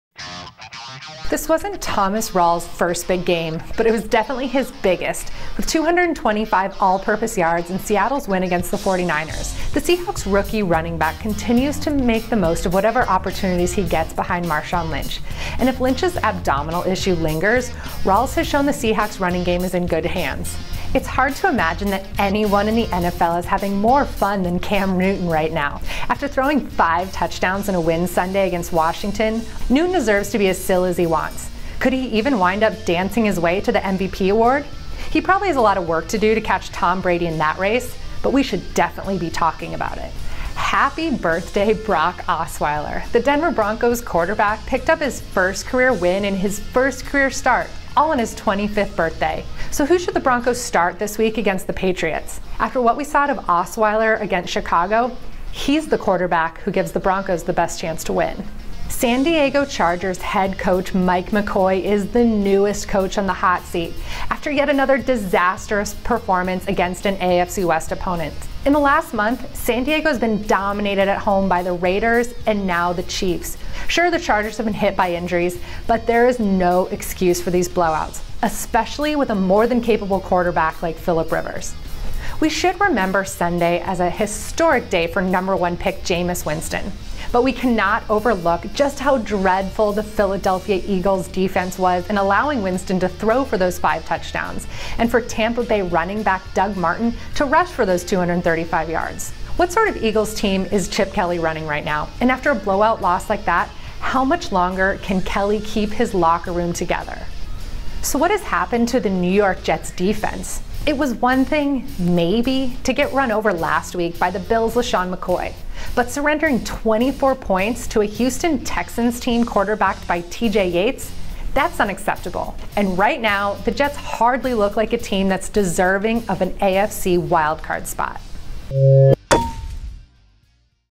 NFL Stock Watch: Rising and Falling entering Week 11
USA Today Sports' Lindsay Jones offers perspective on the biggest storylines from Week 11 and whether or not it's time to panic moving forward.